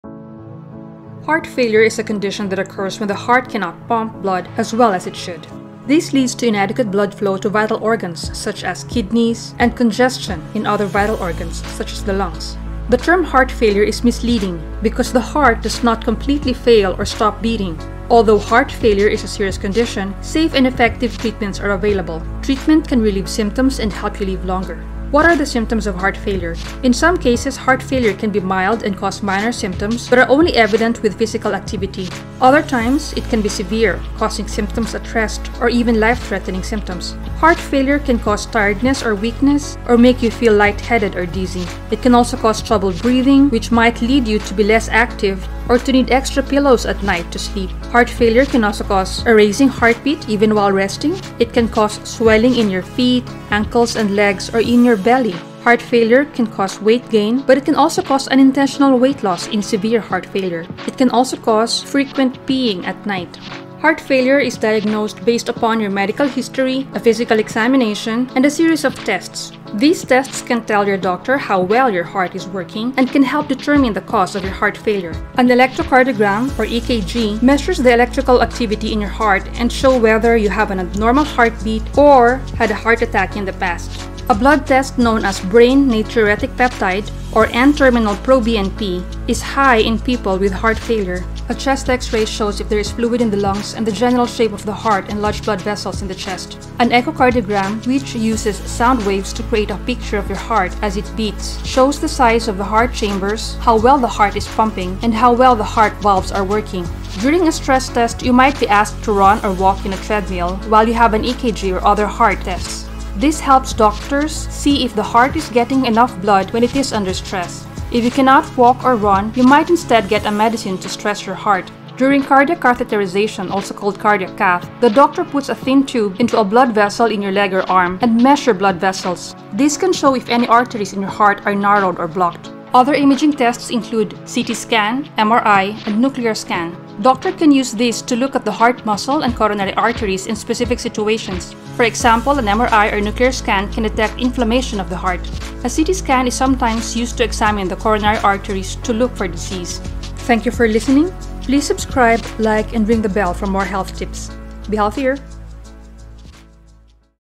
HEART FAILURE Symptoms and Diagnosis
This is an educational video defining heart failure, its symptoms and tests used to diagnose it.

Source: UpToDate 2020